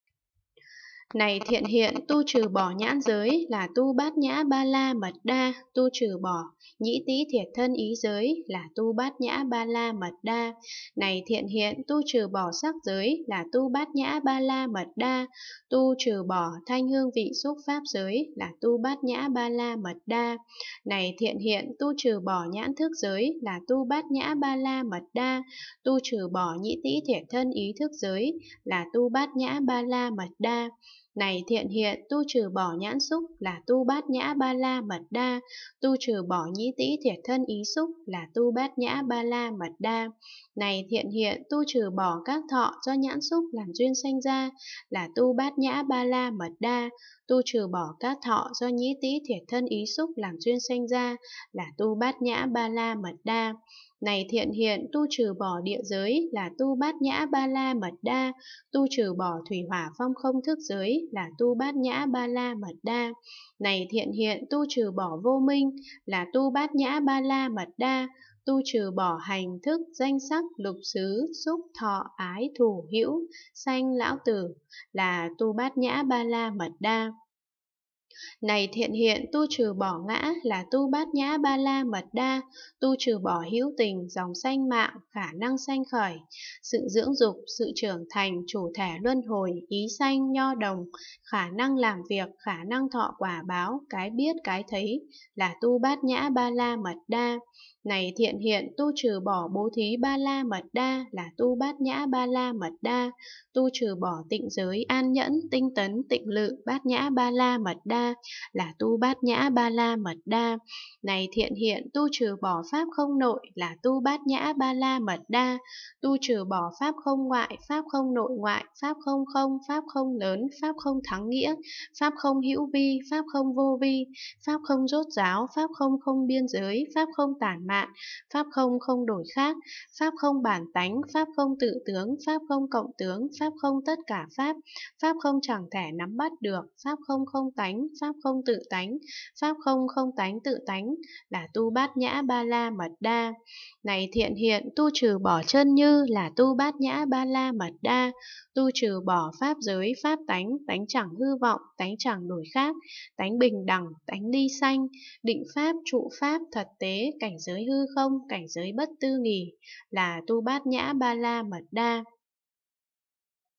Quyển 317: Kinh đại bát nhã ba la mật đa - Phẩm trí hướng đến (rất hay) (317/600) (7)
Phẩm 46: Phẩm trí hướng đến
Hán dịch: Pháp sư Huyền Trang
Việt dịch: Hoà thượng Trí Nghiêm
Giảo chính: Hoà thượng Quảng Độ